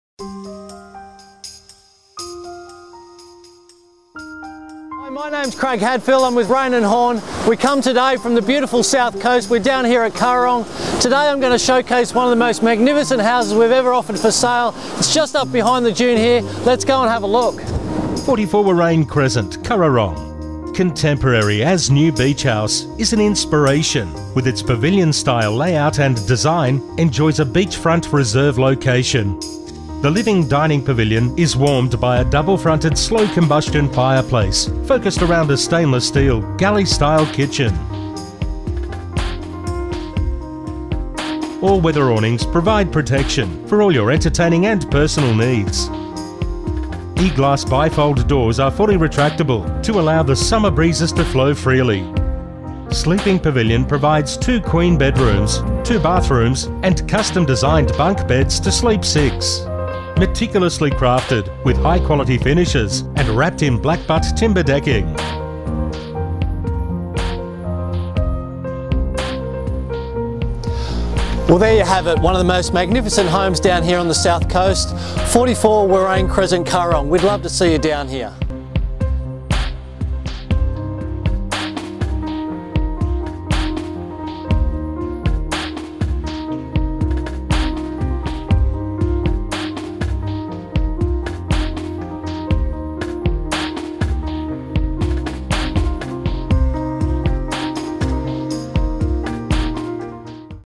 Architectural Design on Beachfront Reserve - sleeps 10
An architectural success, this as-new contemporary beachhouse is an inspiration with it's pavilion style layout and design.  As seen in The Australian Women's Weekly and South Coast Style magazines, the  house provides a warm welcome to your family and friends.  Nestled on a site selected for its level grounds, northern aspect, it enjoys a beachfront reserve location.  Just metres from Warrain Beach.  The house has been meticulously crafted, with high quality finishes and wrapped in Blackbutt timber decking. Painting a striking picture from the street, you will be enthralled by the Peter Nixon designed, maintenance free gardens and Outrigger designed awnings.  The all-weather awnings provide protection for all your entertaining and personal needs.  The picturesque gardens add privacy and tranquillity.  The house offers impressive and spacious interiors.  The living/dining pavilion is warmed by a double-fronted slow combustion fireplace, for  those cozy winter nights.  And during the summer months, the E-glass bi-fold doors are fully retractable to allow the summer breezes to flow freely. Focused around a stainless steel, galley style kitchen, all your cooking and storage needs are met.  The kitchen is functional and equipped with a Bosch oven and cooktop, and integrated dishwasher. The sleeping pavilion provides 2 queen size bedrooms, 2 bathrooms, and custom designed bunk beds to sleep 6.  A laundry and storage room are also located in this area.  The bathrooms are finished in tiles from Artedomus with Parisi bathroomware. Additional features include a carport, large storeroom, Blackbutt  floors, Blindmaster roller blinds,louvred windows, ceiling fans and Italian lighting. This hidden treasure is just a few easy steps along the crystal waters  edge to Shops, cafe, parks and boat ramp.  This really is one of the South Coast's most idyllic hideaways.